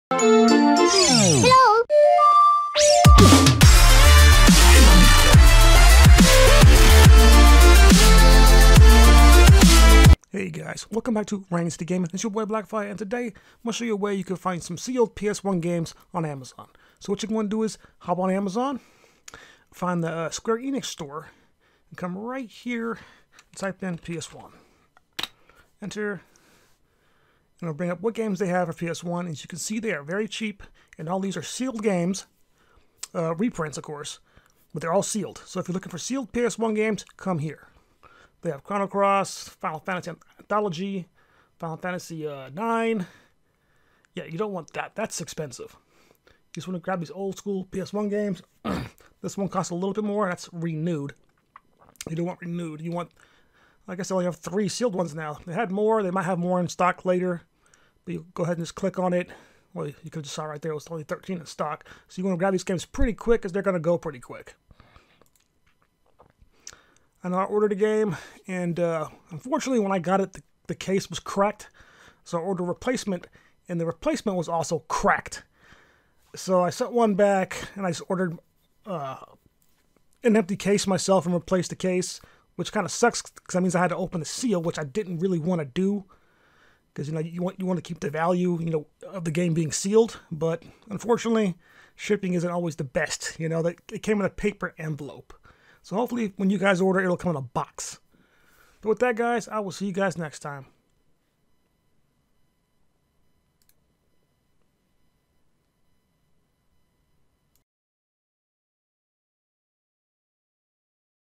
How You Can Find Sealed PS1 Games (Square Enix Games)
To give credit where credit is due, Gameguy1210 gave me the idea to make this video, but sure to check out his channel for even more info on SE game reprints.